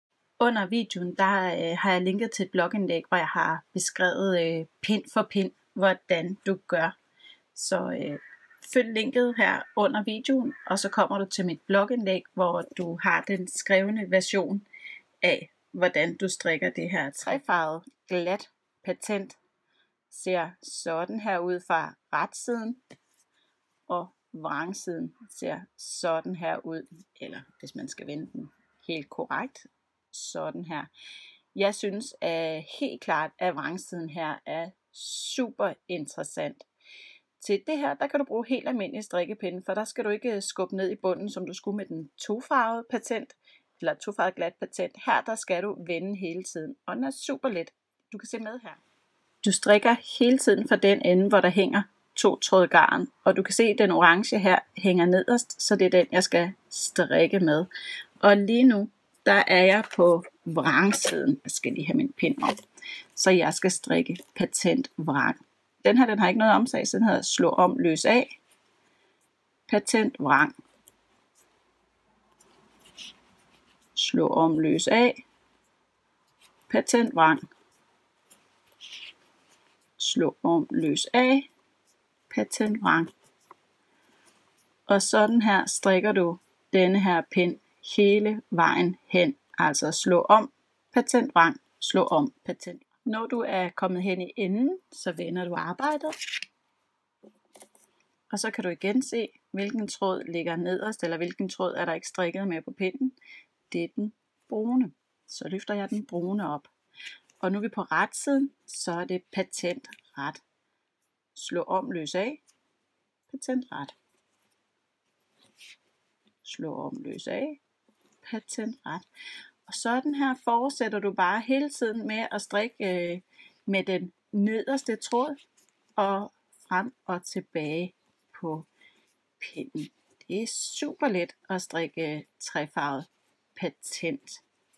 Galt patent 3 farver
glat patent strik med 3 farver er faktisk den letteste at strikke når det kommer til patent mønstre med 2 eller flere farver.